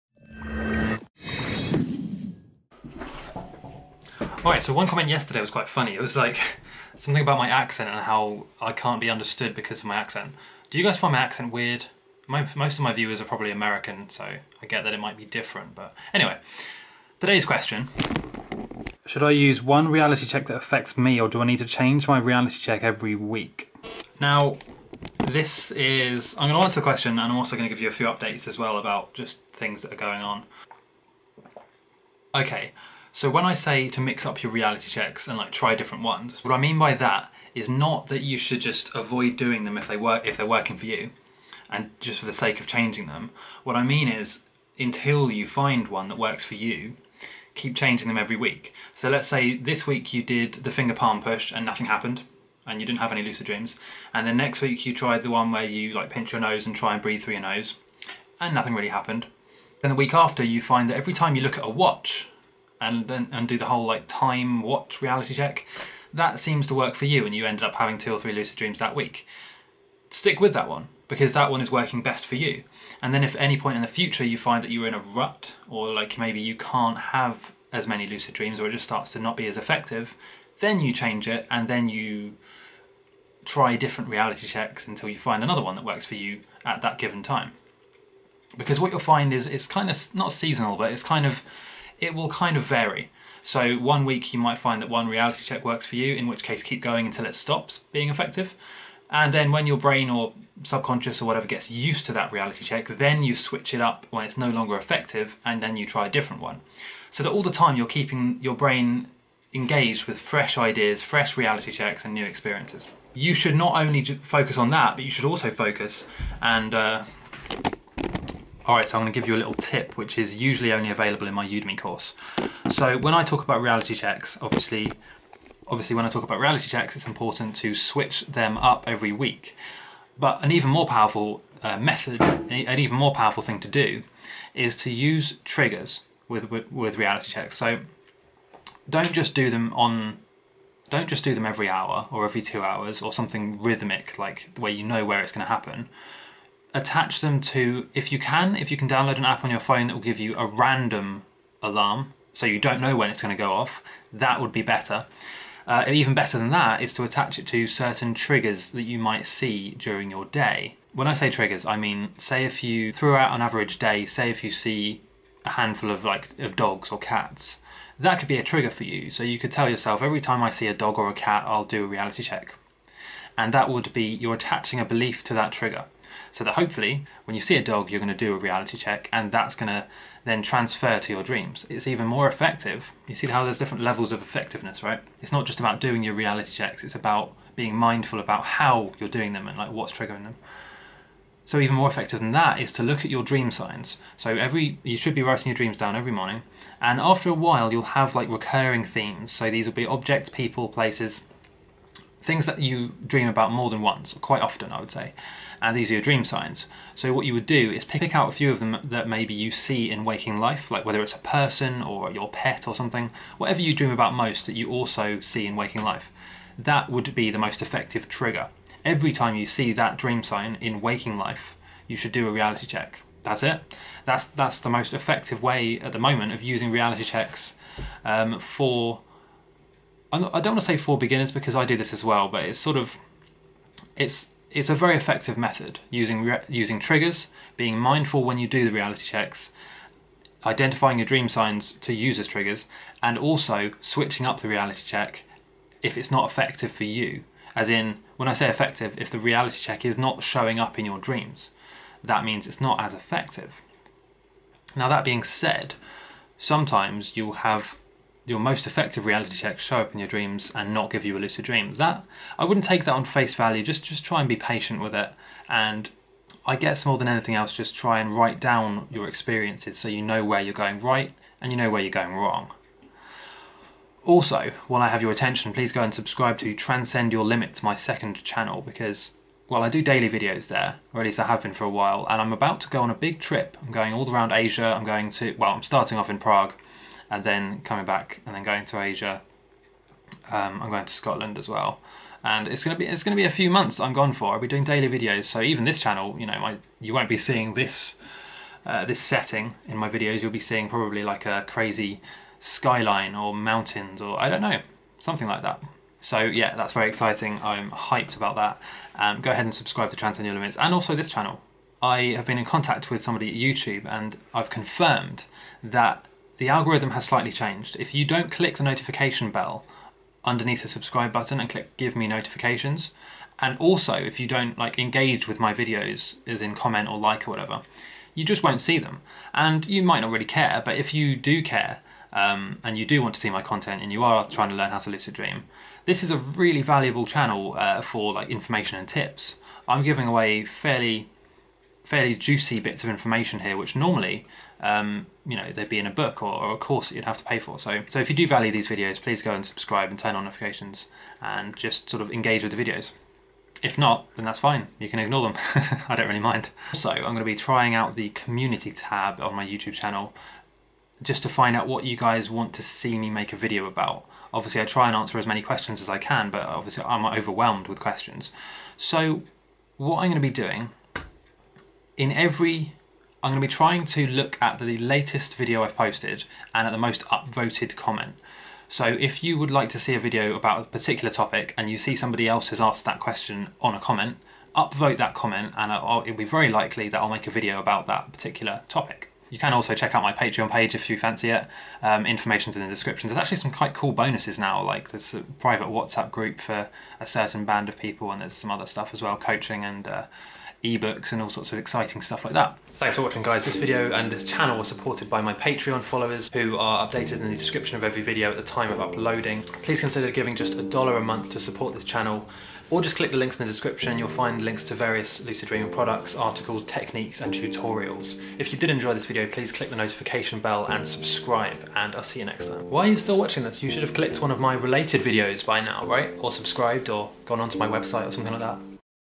The TRUTH About Reality Checks For Lucid Dreaming
✨ This is what will make the difference between someone who does reality checks all the time and has NO lucid dreams, and someone who does a few reality checks a day and has LOTS of natural lucid dreams. Reality testing is a very common technique for lucid dreaming and you should really focus on this video just for a few minutes!